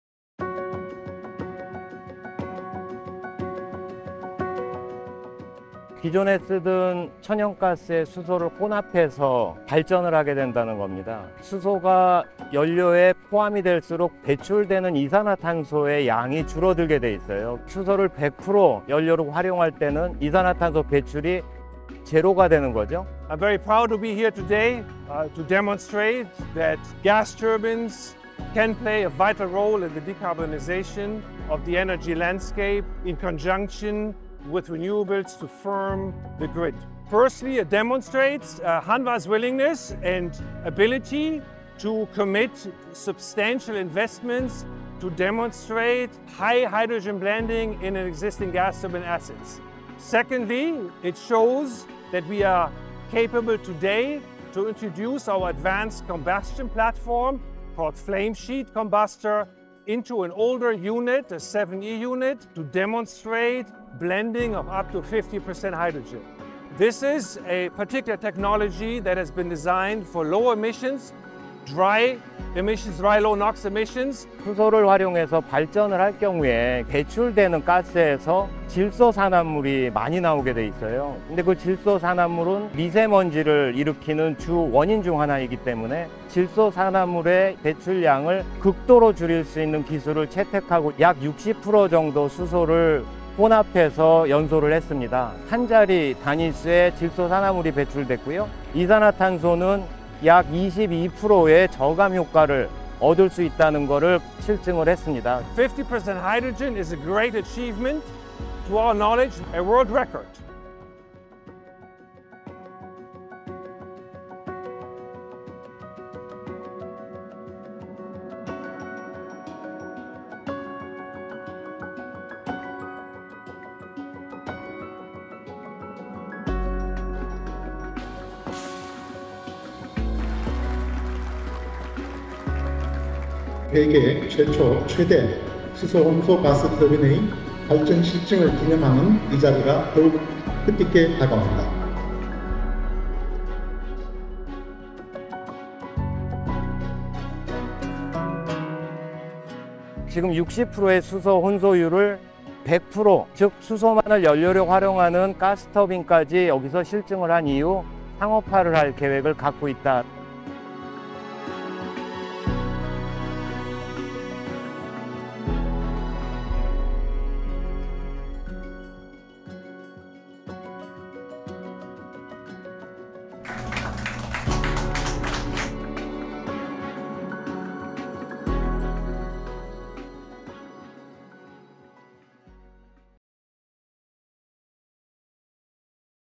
Hanwha Sets New World Record for Hydrogen Co-firing Gas Turbine
Climate change is causing unprecedented changes and challenges, and solutions to decarbonize the economy are greatly needed. Hydrogen gas turbine technology holds great promise for decarbonizing hard to abate industries and preventing stranded assets as the clean energy transition progresses. Hanwha recently achieved a world record by operating an 80-MW class turbine with a hydrogen blend of 60%. Watch to learn more about hydrogen gas turbine technology and how Hanwha is leading the charge in developing this important bridge technology.

00:00 What is hydrogen gas turbine technology?
00:24 Hanwha’s world record
01:14 Hanwha’s hydrogen gas turbine technology